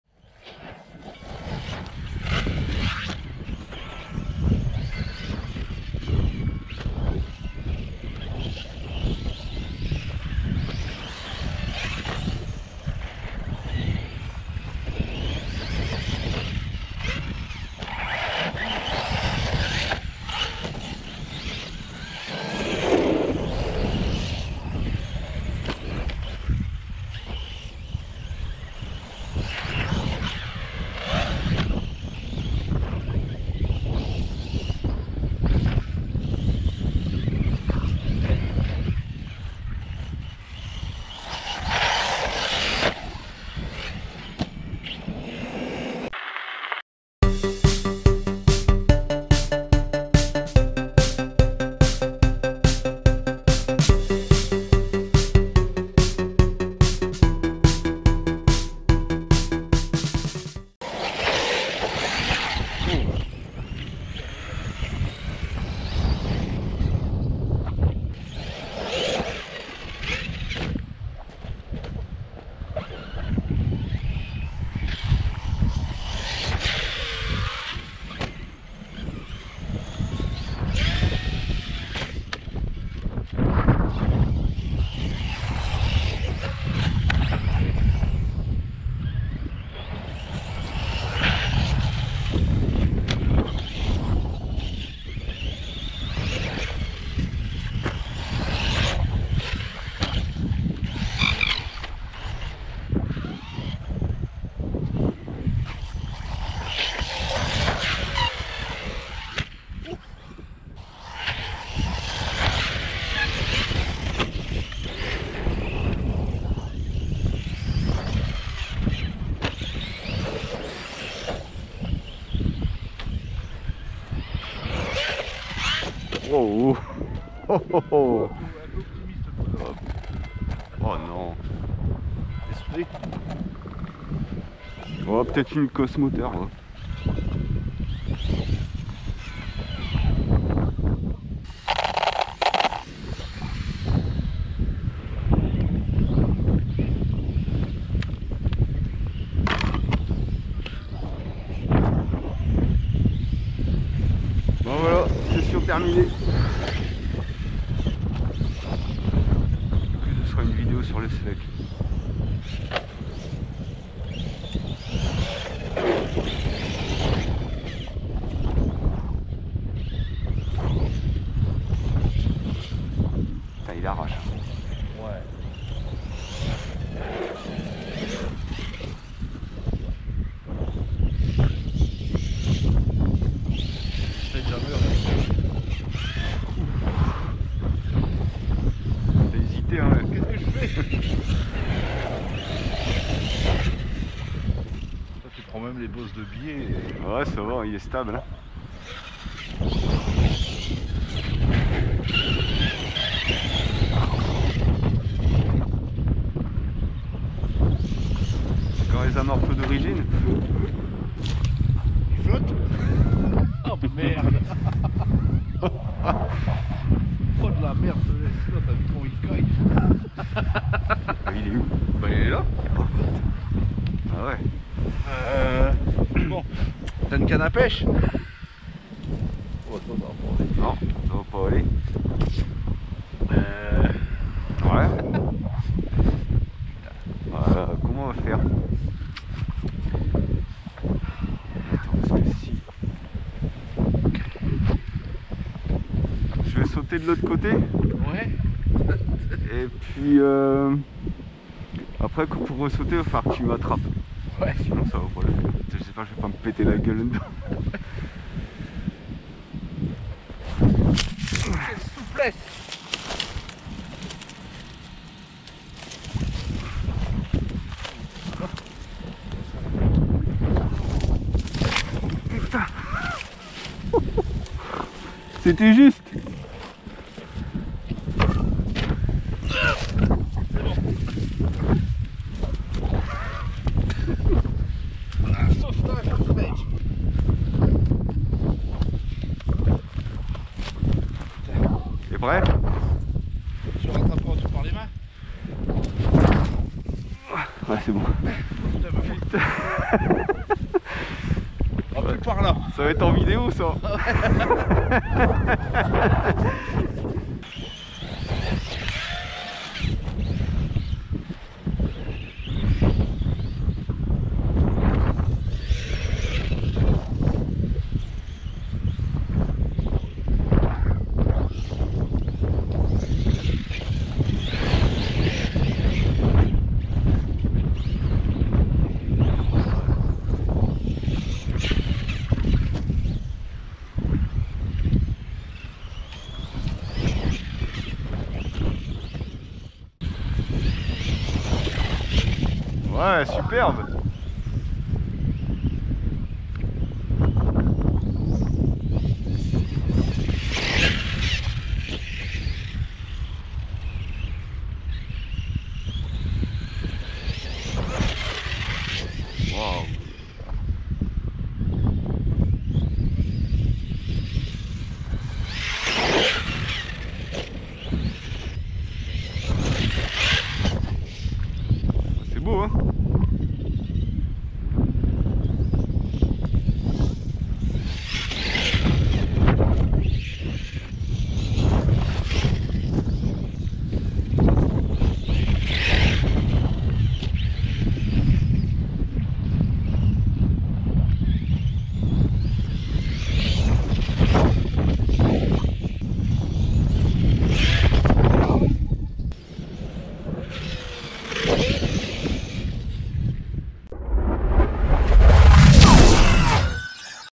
outcast et sledge qui prend l'eau
après un peu plus de 2 mois sans sortie le outcast sort pour une session avec le sledge.
moteur hs pour le outcast ( moteur hybride mi castle mi surpass hobby) et un petit bain pour le sledge ...